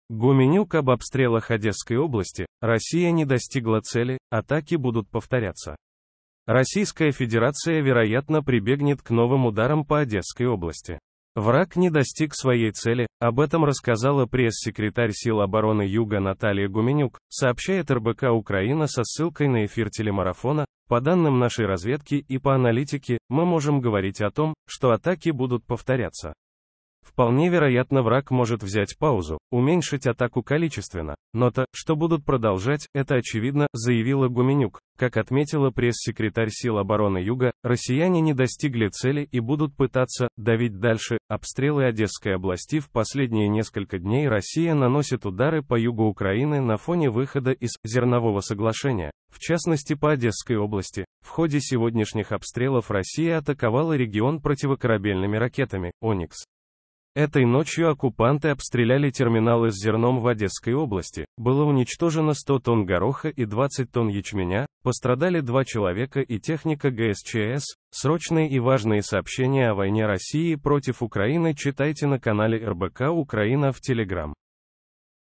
Гуменюк об обстрелах Одесской области: Россия не достигла цели, атаки будут повторяться.
ВойнавУкраине Одесса Российская Федерация вероятно прибегнет к новым ударам по Одесской области. Враг не достиг своей цели. Об этом рассказала пресс-секретарь Сил обороны юга Наталья Гуменюк, сообщает РБК-Украина со ссылкой на эфир телемарафона. "По данным нашей разведки и по аналитике, мы можем говорить о том, что атаки будут повторяться. Вполне вероятно враг может взять паузу, уменьшить атаку количественно, но то, что будут продолжать - это очевидно", - заявила Гуменюк. Как отметила пресс-секретарь Сил обороны юга, р